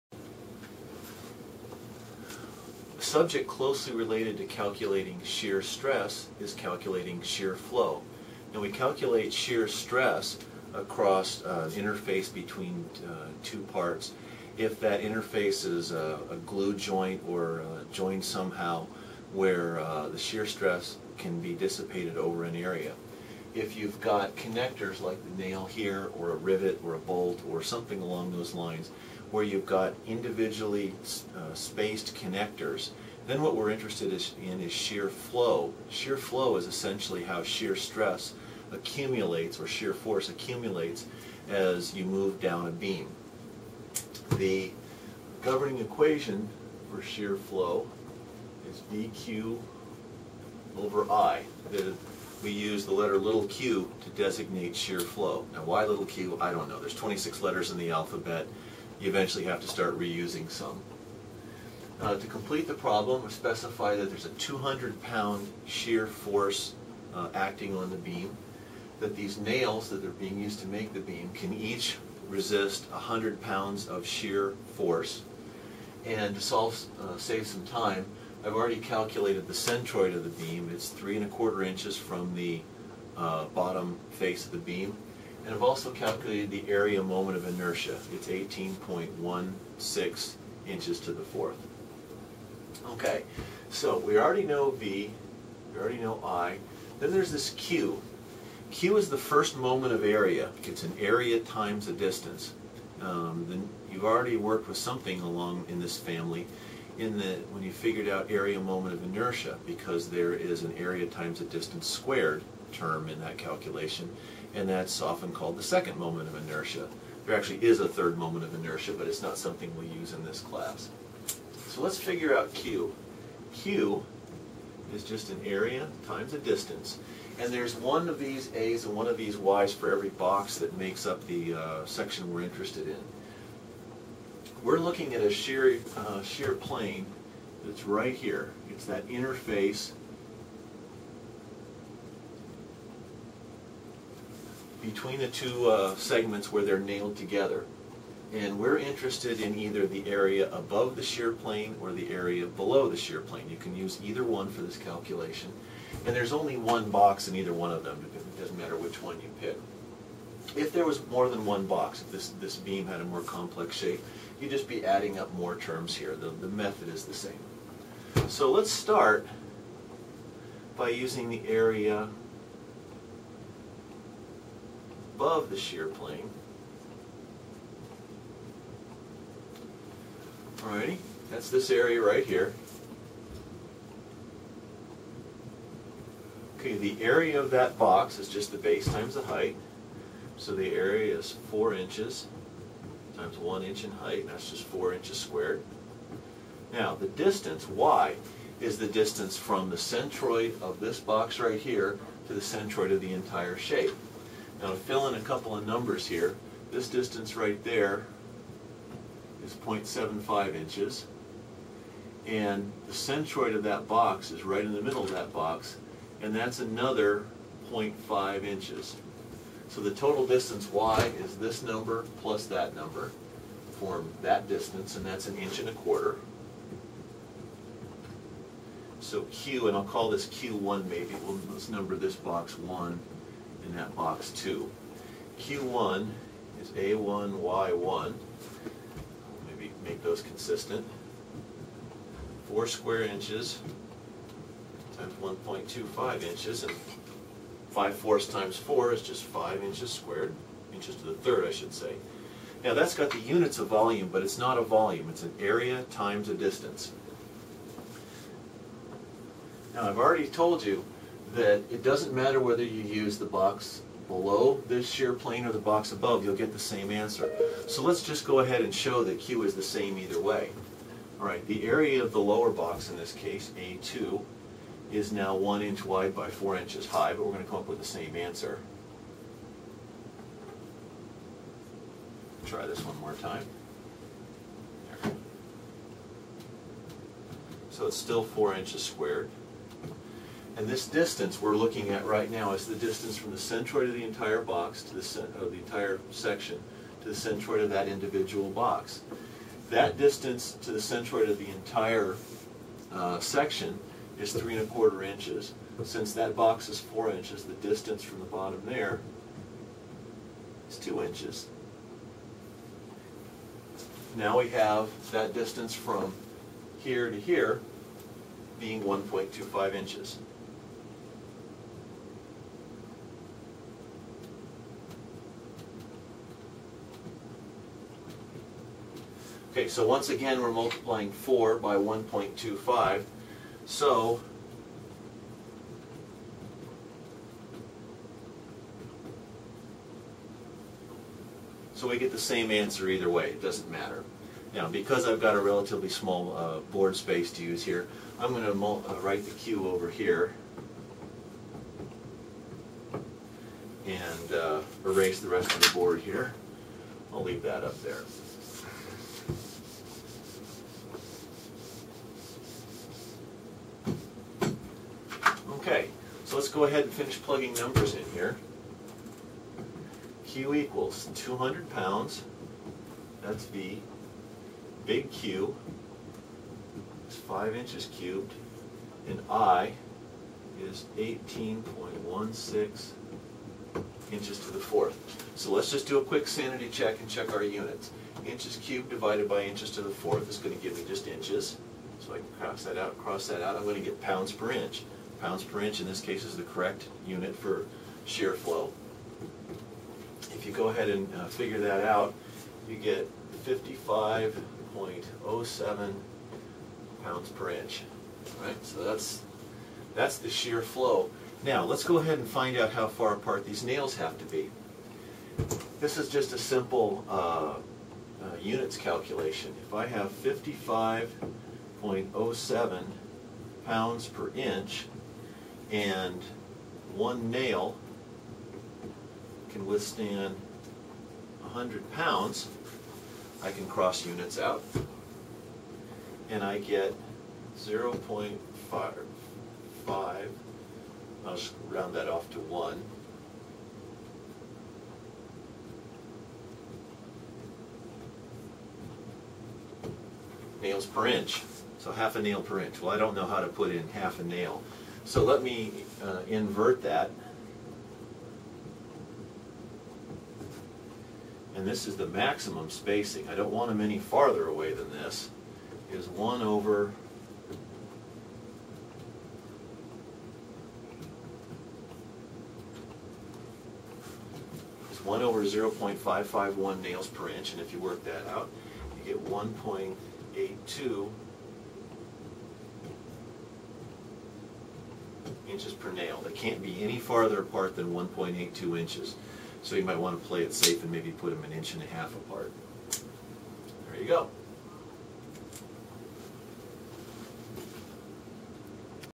Shear Flow in T Beam.MP4
Example showing how to calculate shear flow and connector spacing in a T-shaped beam